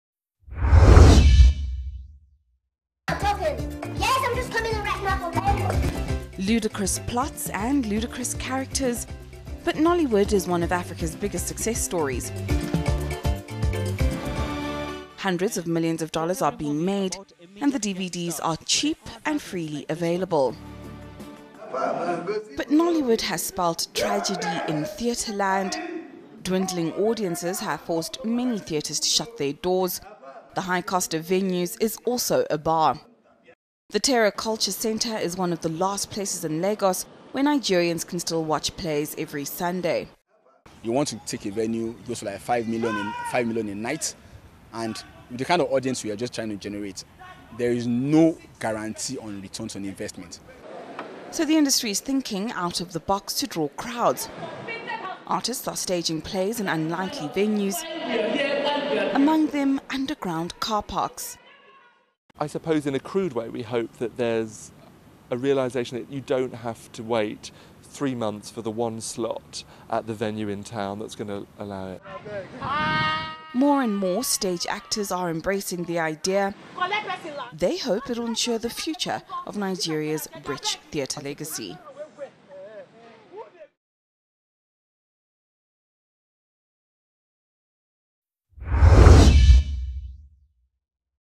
Nigerian actors avoid final curtain
Theatre in Nigeria has played a key role in the country's history but more recently it is struggling to find an audience. Now some young actors have come up with dramatic plans to avoid the final curtain.